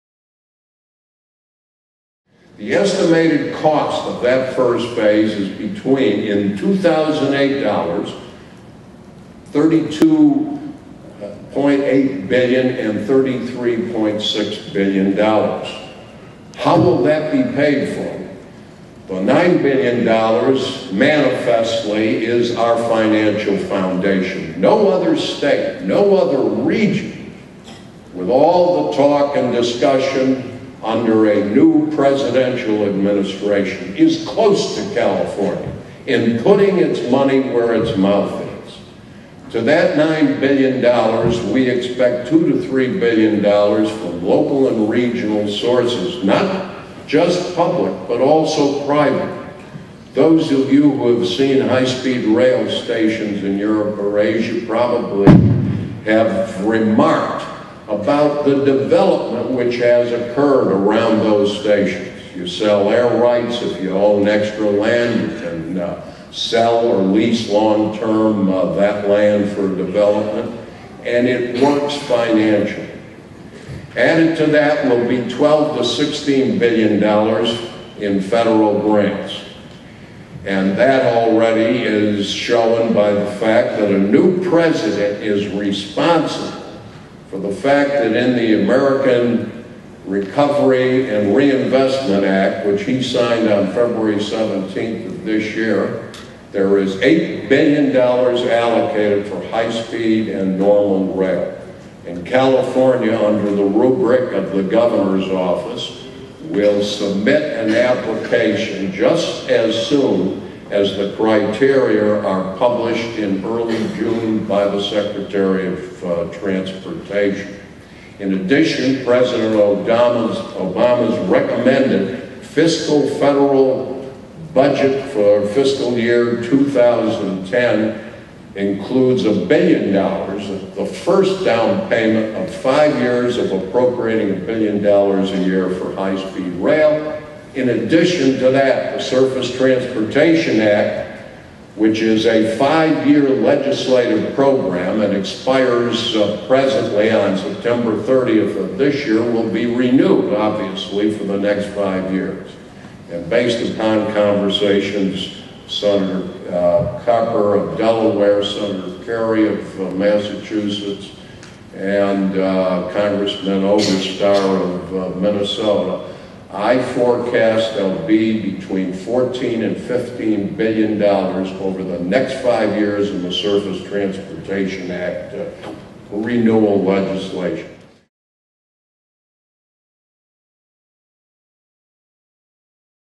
Quentin Kopp Speaks on Funding High Speed Rail
Quentin Kopp speaks at the Great Valley Center Conference on May 6, 2009 about High Speed Rail in the Central Valley.